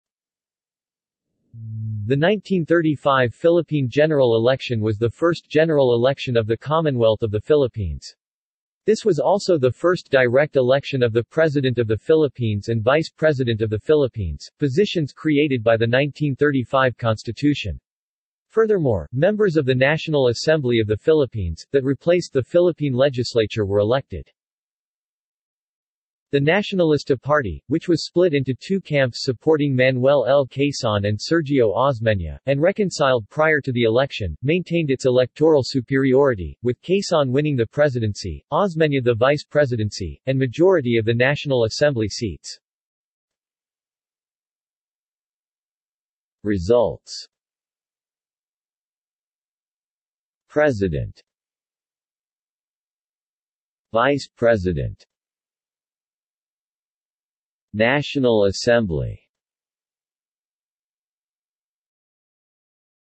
Philippine general election, 1935 | Wikipedia audio article
This is an audio version of the Wikipedia Article:
Philippine general election, 1935

SUMMARY
The 1935 Philippine general election was the first general election of the Commonwealth of the Philippines. This was also the first direct election of the President of the Philippines and Vice President of the Philippines, positions created by the 1935 constitution. Furthermore, members of the National Assembly of the Philippines, that replaced the Philippine Legislature were elected.
The Nacionalista Party, which was split into two camps supporting Manuel L. Quezon and Sergio Osmeña, and reconciled prior to the election, maintained its electoral superiority, with Quezon winning the presidency, Osmeña the vice presidency, and majority of the National Assembly seats.